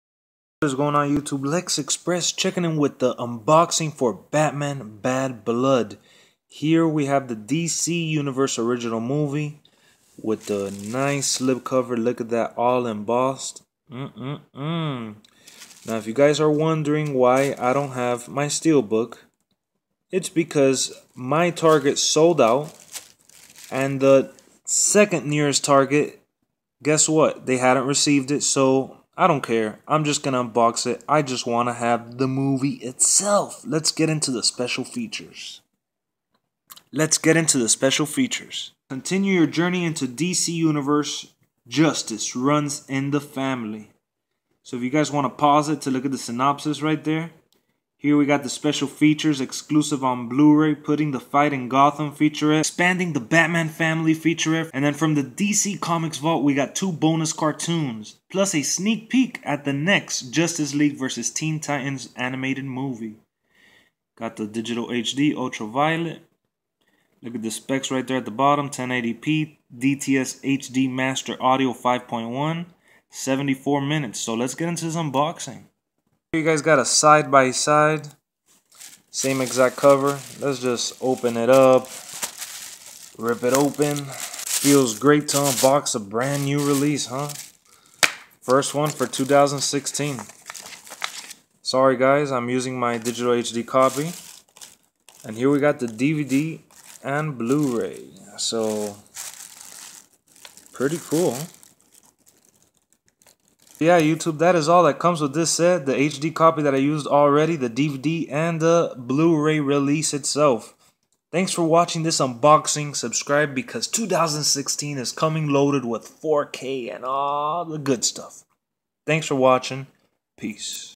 Batman Bad Blood Bluray UNBOXING - DC Comics Anime
here is the latest batman anime movie by dc comics! check out this awesome unboxing!

Batman: Bad Blood (2016)
PG-13 | 1h 12min | Animation, Action, Sci-Fi
Bruce Wayne is missing. Alfred covers for him while Nightwing and Robin patrol Gotham City in his stead. And a new player, Batwoman, investigates Batman's disappearance.
Director: Jay Oliva
Writers: J.M. DeMatteis, Grant Morrison (comic book)
Stars: Jason O'Mara, Yvonne Strahovski, Stuart Allan